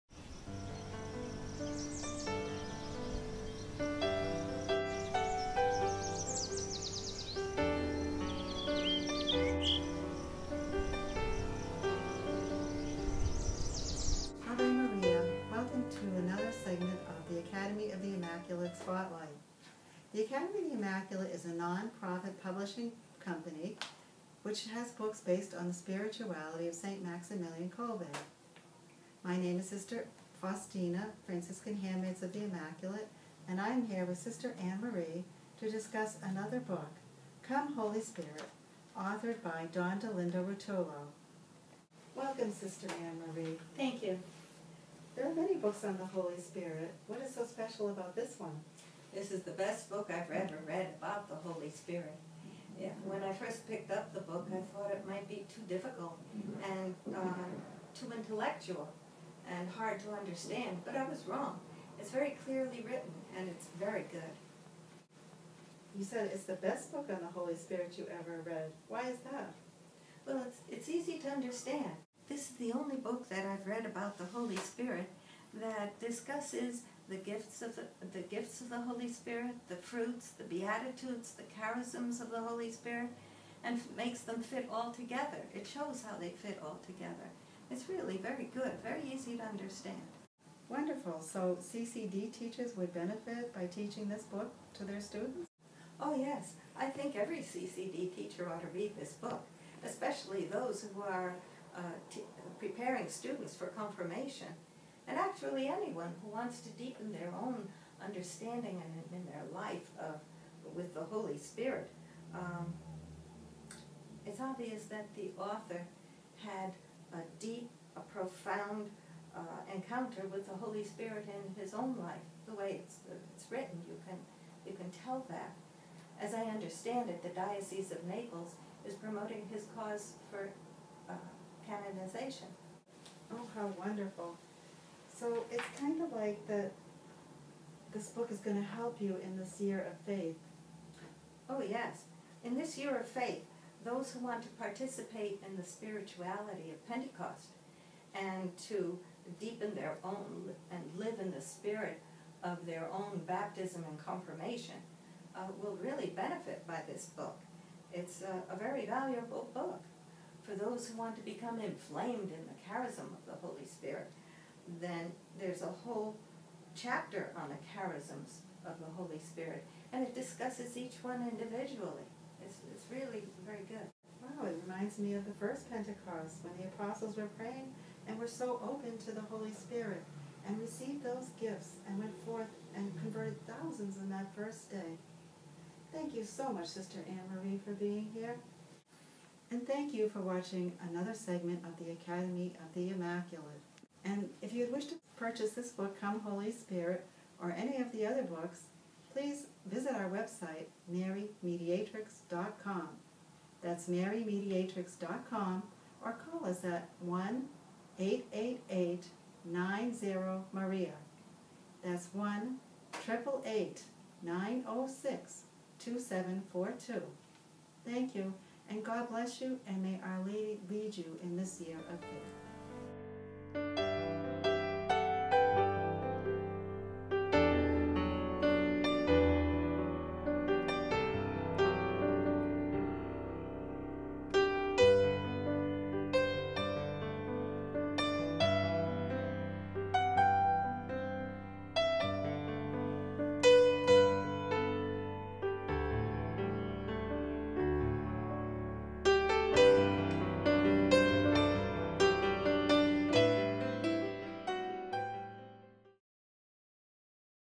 "Come Holy Spirit" by Academy of the Immaculate
This is a brief book review/interview presented by the Franciscan Handmaids of the Immaculate on the book "Come, Holy Spirit" by Don Dolindo Ruotolo. The book was published by the Academy of the Immaculate (marymediatrix.com).